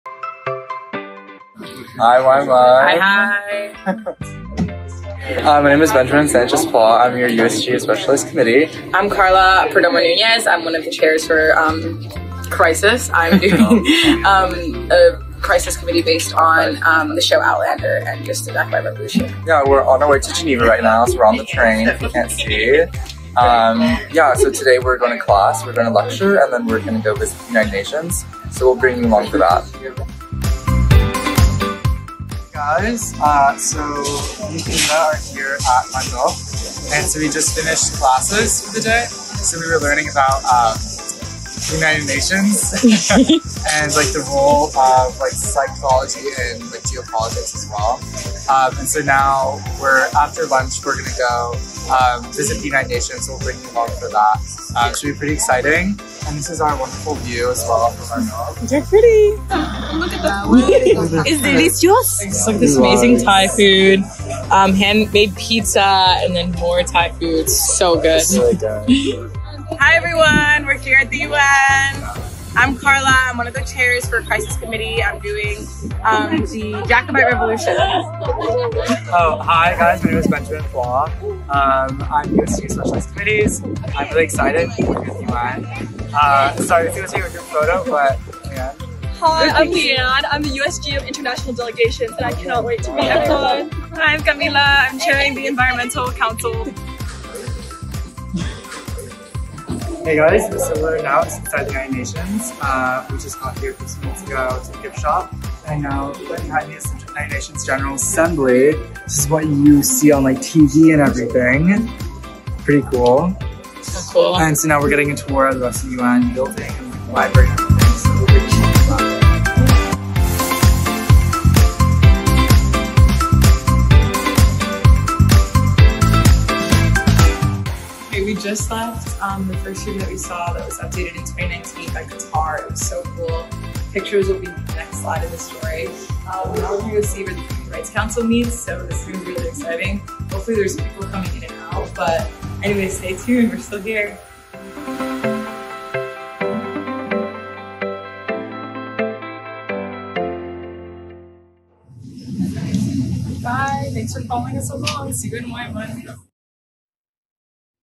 Yale Students in Switzerland - Summer Secretariat Series (Episode 1)
Come along and travel with YMUN this summer! First stop - Geneva, Switzerland!